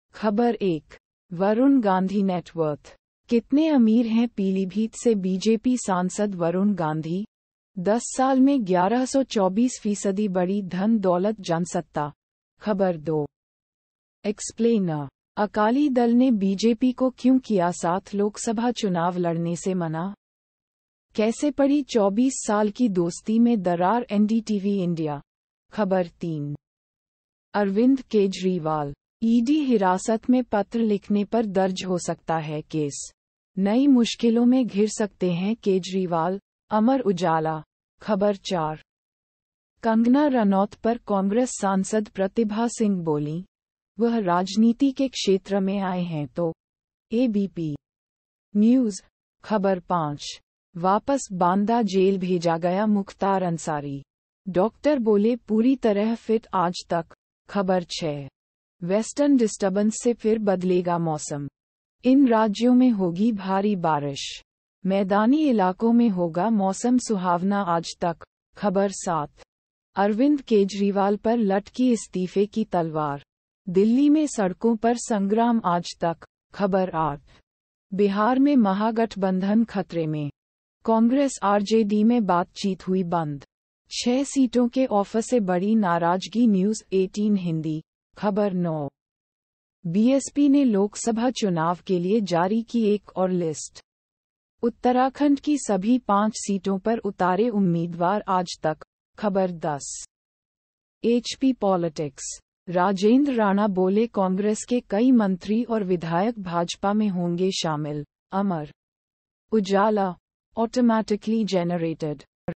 Hindi News (भारत)- 26/03/2024,10:09 PM GMT+5:30
ख़बर 1:Varun Gandhi Net Worth: कितने अमीर हैं पीलीभीत से BJP सांसद वरुण गांधी? 10 साल में 1124 फीसदी बढ़ी धन-दौलत - Jansatta
ख़बर 2:Explainer : अकाली दल ने BJP को क्यों किया साथ लोकसभा चुनाव लड़ने से मना? कैसे पड़ी 24 साल की दोस्ती में दरार - NDTV India
ख़बर 3:Arvind Kejriwal: ईडी हिरासत में पत्र लिखने पर दर्ज हो सकता है केस, नई मुश्किलों में घिर सकते हैं केजरीवाल - अमर उजाला
ख़बर 4:कंगना रनौत पर कांग्रेस सांसद प्रतिभा सिंह बोलीं- 'वह राजनीति के क्षेत्र में आई हैं तो...' - ABP न्यूज़
ख़बर 5:वापस बांदा जेल भेजा गया मुख्तार अंसारी, डॉक्टर बोले- पूरी तरह फिट - Aaj Tak
ख़बर 6:वेस्टर्न डिस्टरबेंस से फिर बदलेगा मौसम! इन राज्यों में होगी भारी बारिश, मैदानी इलाकों में होगा मौसम सुहावना - Aaj Tak
ख़बर 7:अरविंद केजरीवाल पर लटकी इस्तीफे की तलवार, दिल्ली में सड़कों पर संग्राम - Aaj Tak
ख़बर 9:BSP ने लोकसभा चुनाव के लिए जारी की एक और लिस्ट, उत्तराखंड की सभी 5 सीटों पर उतारे उम्मीदवार - Aaj Tak
ख़बर 10:HP Politics: राजेंद्र राणा बोले- कांग्रेस के कई मंत्री और विधायक भाजपा में होंगे शामिल - अमर उजाला

Automatically Generated...